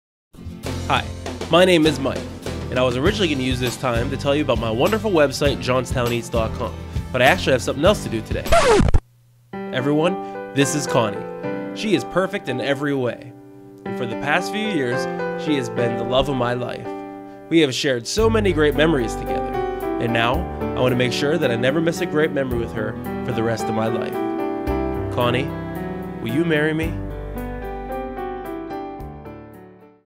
Johnstown Eats | Proposal October 2012 | Commercial | Blink Multimedia
On Wednesday October 24th durring the Ellen Show on WJAC, Johnstown Eats ran it's first televised commercial. Only it wasn't a normal commercial by any means. Mike Cameron, the owner of Johnstown Eats actually used that time to propose to his beautiful girlfriend Connie...

By the way ... She said YES!